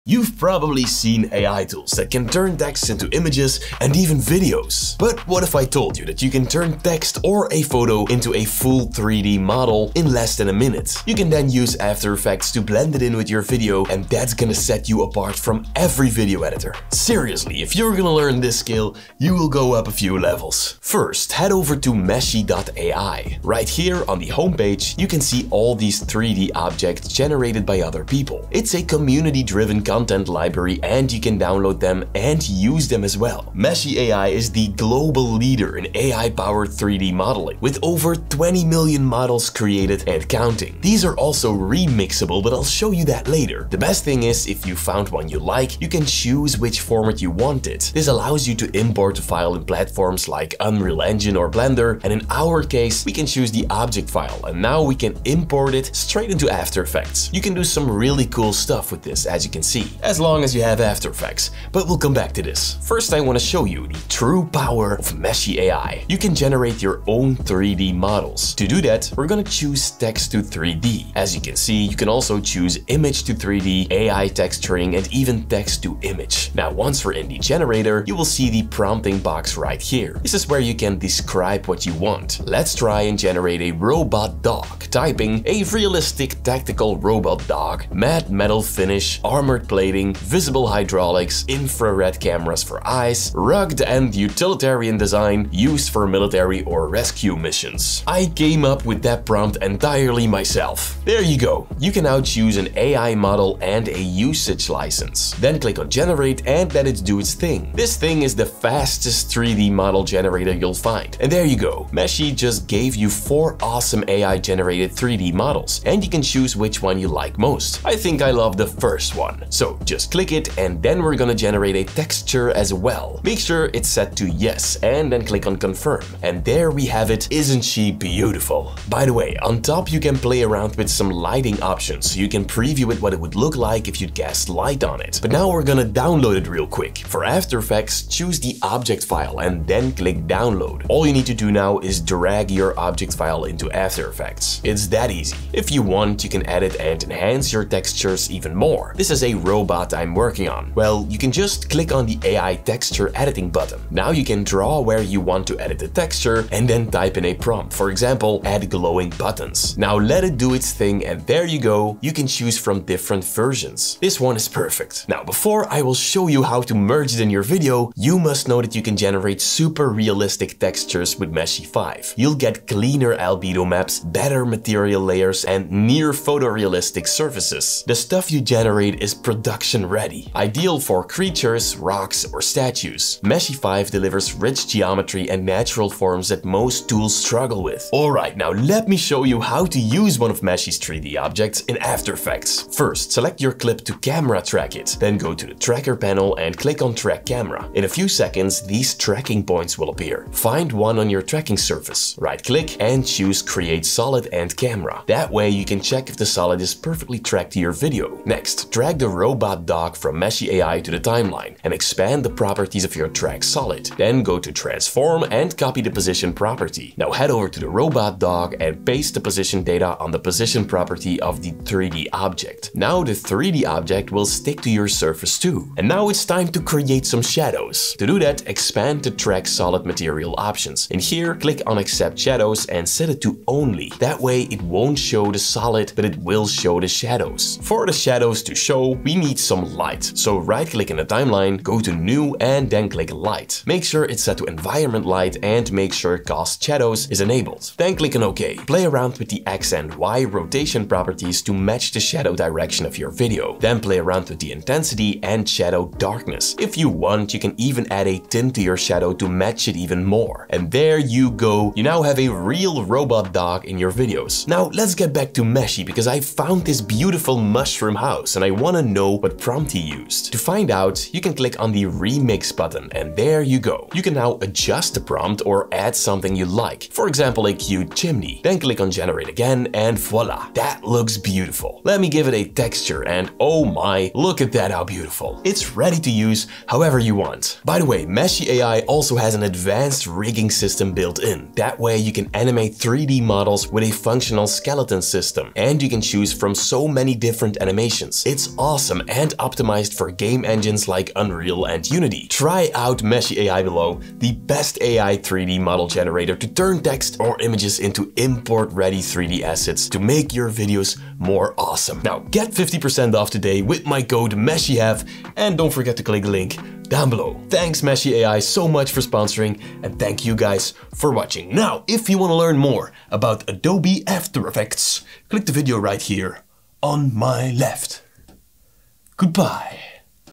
Turn ANY IMAGE Into a 3D Object (Meshy + AE Tutorial)
(advertising @MeshyAI)❤️Try out Meshy AI — the best AI 3D model generator to turn text or images into game - ready 3D assets in minutes!